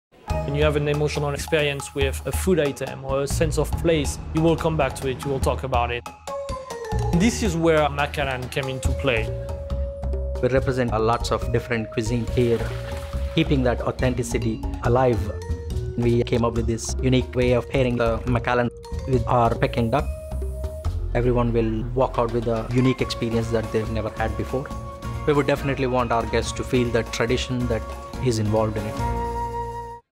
Macallan's World's Best Scotch Experiences at The Setai, Miami Beach
Selected to develop and host one of The World's Best Scotch Experiences, The Setai's Executive Chef Vijay Veena and Food & Beverage Director Julien Labays have collaborated with Macallan to create an unparalleled tasting menu. Look for continued elements of this partnership to develop over the year.